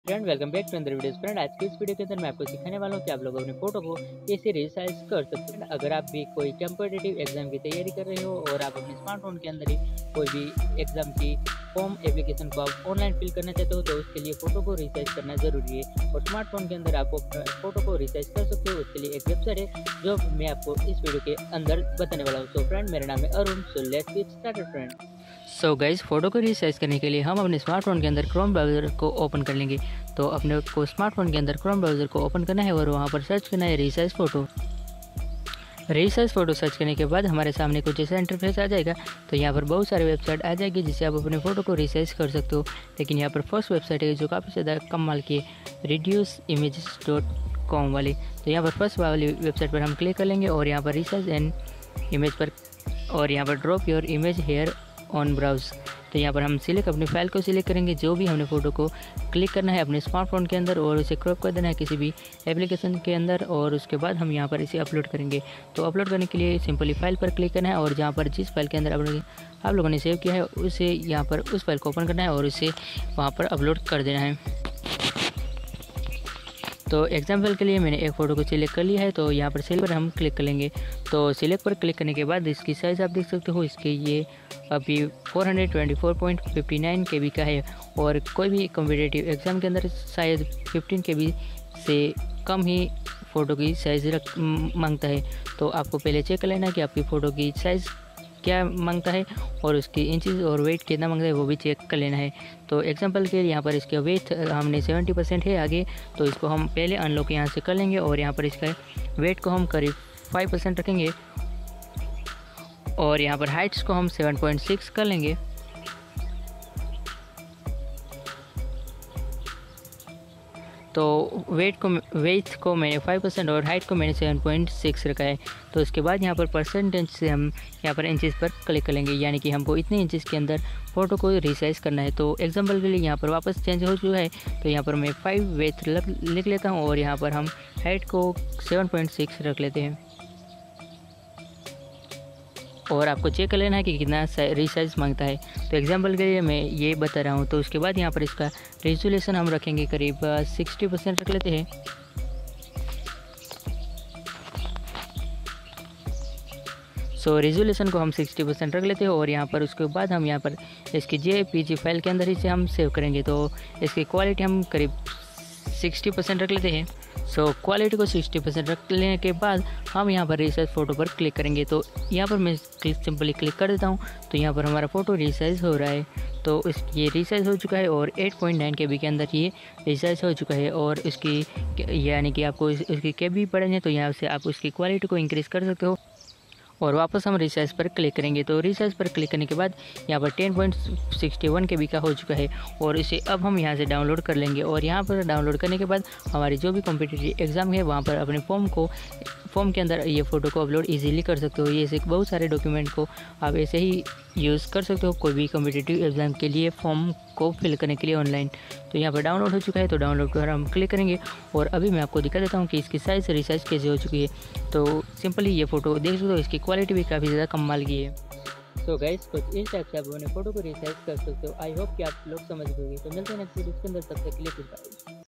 How To Resize Your PHOTO Online
How To Resize Your PHOTO Online | Photo Ko Resize Kese Kare | Resize Images

Hello Guy's Aj Ki Is Video Me Mene Apko Sikhya Hai Ki Ap Apne Photo Ko Online Resize Kaise Kar Sakte Hai | Ap Bhi Kisi Bhi Competitive Exam Ka Online Form Fill Karna Chate Hai To Photo Ko Resize Karna Jaruri Hai is ke liye Mene Apko Photo Ko Resize Karna Sikhya Hai jisse Ap Apne Smartphone Se Hi Online form Fill Kar Sakte Ho .

I Am From Gujarat